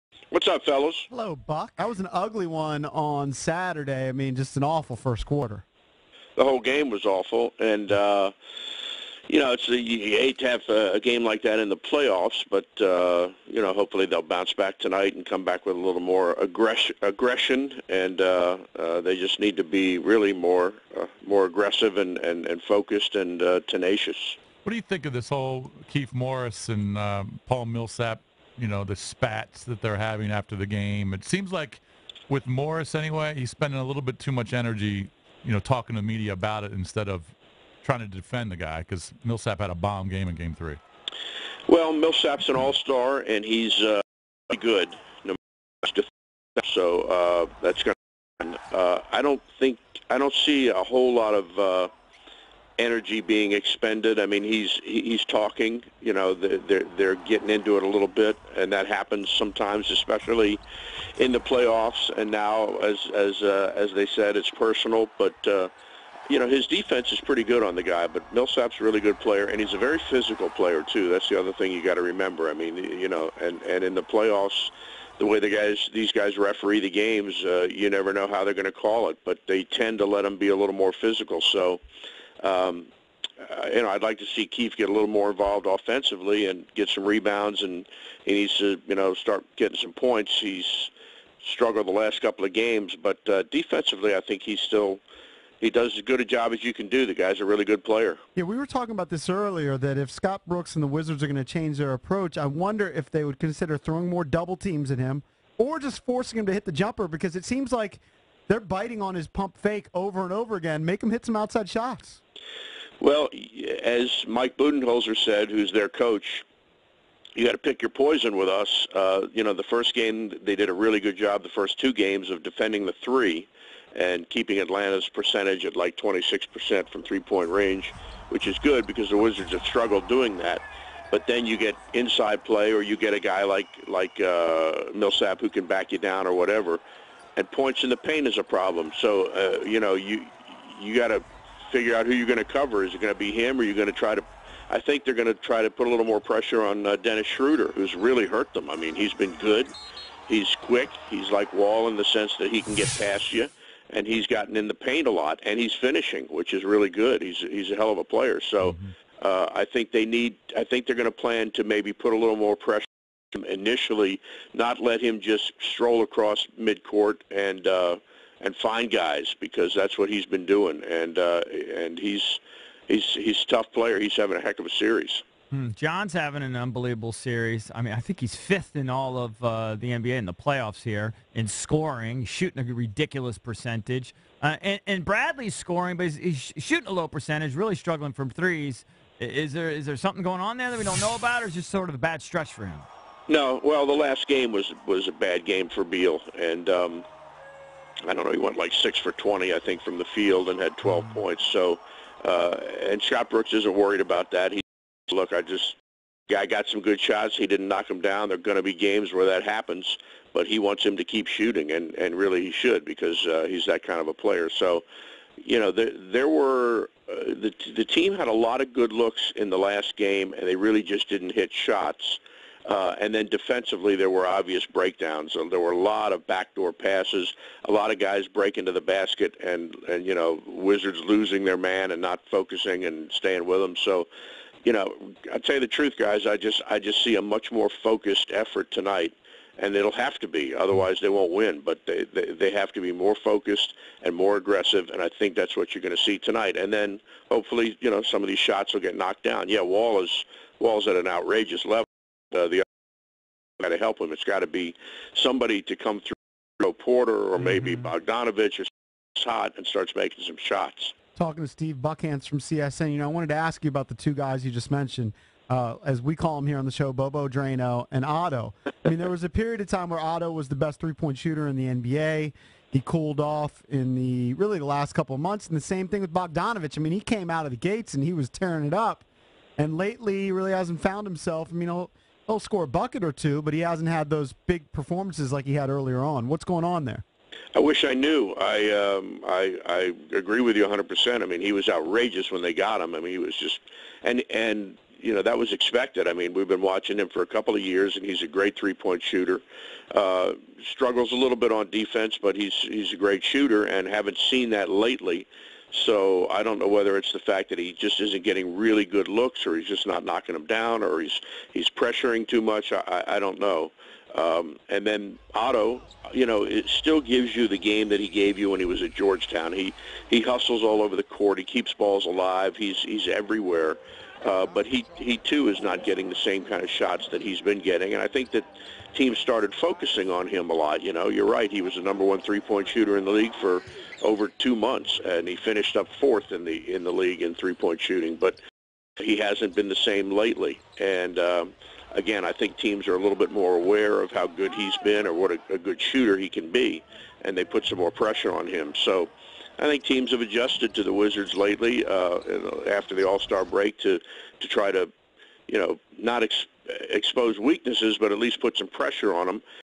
Buckhantz: Keys for Wizards in Game 4
CSN Wizards Play-by-play announcer Steve Buckhantz, breaks down the Wizards VS Hawks Game 4 tonight.